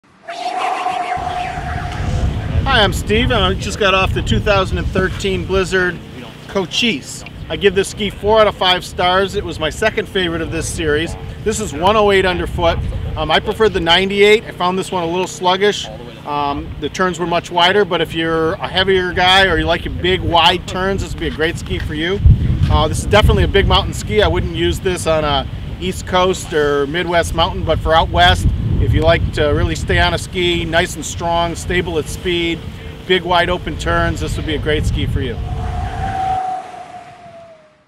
2013 Blizzard Cochise Ski Review By Skis.com
The Blizzard Cochise wasn't ski of the year for no reason. The extremely versatile shape is best suited for the ungroomed powder, bowls and trees, but can take on the rest of the mountain with ease. At 108 mm underfoot the Cochise can float in all but the deepest snow, and still be a riot to rail down groomers. Blizzards Flipcore 3D builds the rocker in the ski naturally without bending or breaking the wood core. The rocker in the tip and tail make the Cochise very playful, yet stable enough for you test your speed limit while still making the ski easy to float in all of the deep stuff. A layer of titanium adds some torsional and lateral strength, but a layer of bamboo keeps the ski light and easy to maneuver in tight conditions. Vertical sidewalls add to the toughness of the Cochise and make it easy to grip on firm snow when some goon has scratched off all of the snow at the top of the chute. If you are looking for a ski that loves to be on anything ungroomed and covered with snow, the Blizzard Cochise fits the bill.